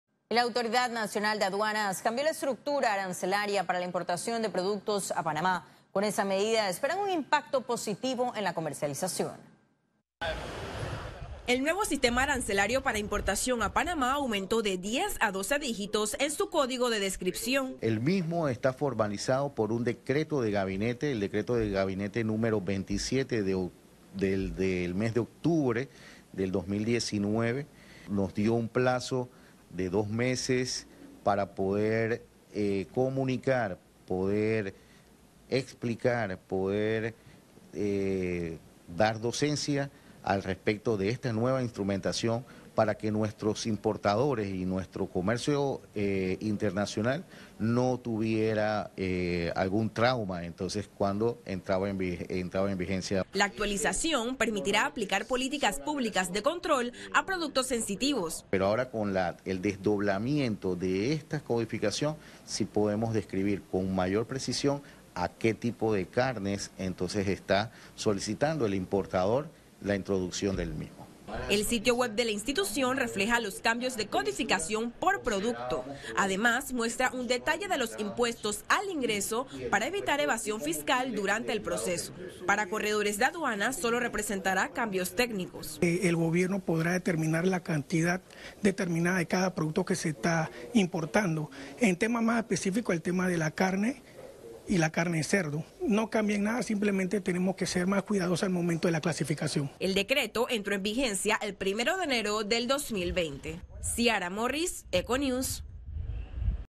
Aduanas cambió sistema arancelario para importación | ECO News
La Autoridad Nacional de Aduanas cambió la estructura arancelaria para la importación de productos a Panamá. Con esta medida esperan un impacto positivo en la comercialización.